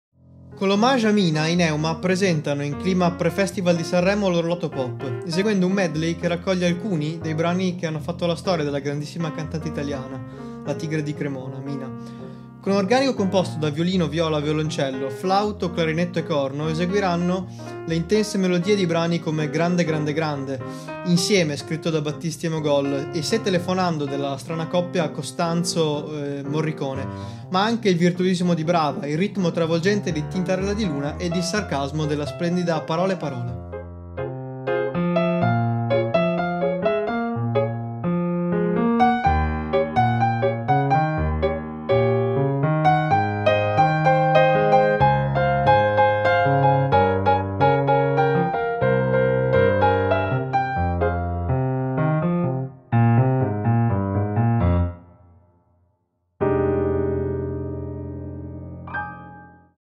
Hommage à Mina - weareneuma​ Festival 4/4
Ensemble Neuma presenta la prima edizione del weareneuma​​​ Festival.
Ogni domenica di febbraio 2021 alle ore 20:00 l'ensemble propone un concerto cameristico sui propri canali online. Domenica 28 febbraio il concerto Hommage à Mina, un omaggio alla "Tigre di Cremona"

Prendi nota degli appuntamenti, ti aspettiamo!
7/02 Les petits nerveux
14/02 Soliloquio Neuma
21/02 Matadores Neuma
28/02 Hommàge à Mina